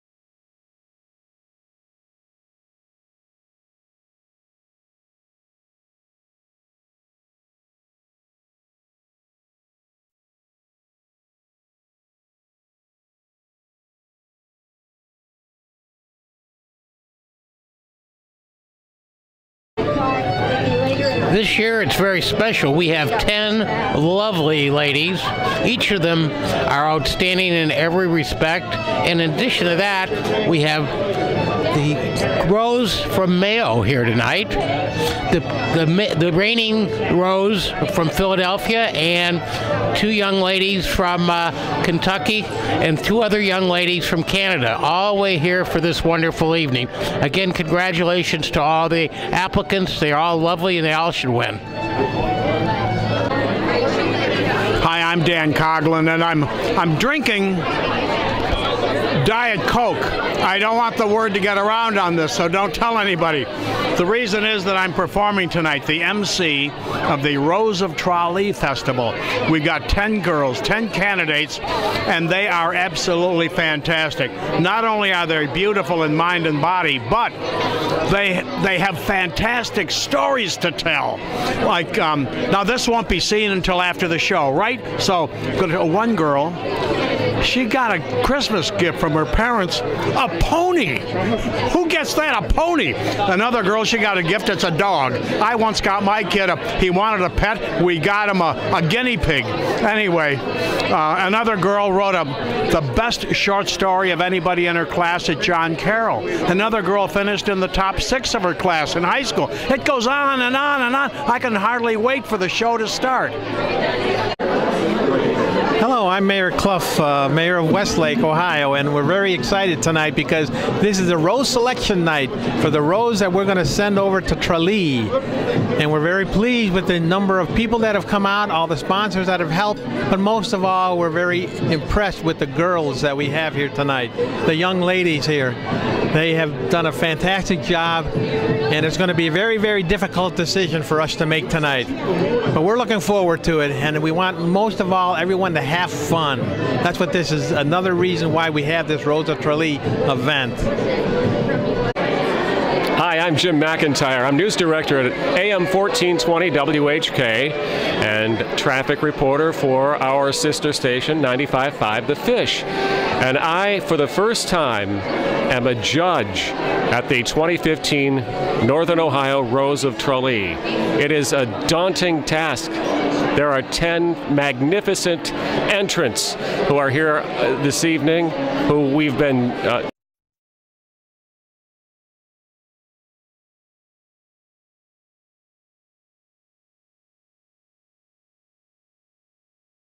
Tom Scanlon, Dan Coughlin, Dennis Clough, Jim McIntyre 2
2015 Ohio Rose of Tralee Selection Celebration
West Sidd Irish American Hall
Cleveland, Ohio